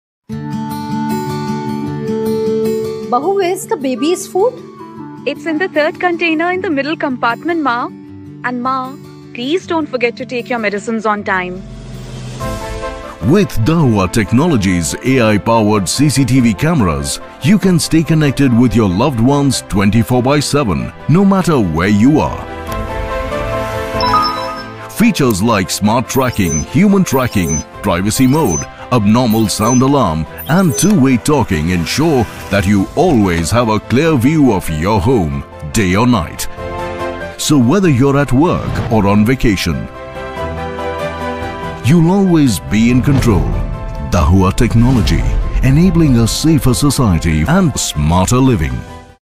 Dahua IMOU Ranger 2 Wifi Camera
1080P H.265 Wi-Fi Pan & Tilt Camera With 1080P Full HD live monitoring and 0~355° pan & -5~80°tilt features, Ranger 2 ensures every corner of your home completely covered. Human Detection quickly finds human targets in images and immediately sends a notification to you. Privacy Mode helps protect your personal privacy when you are home, and Smart Tracking function lets the camera follows moving objects when it detects motion.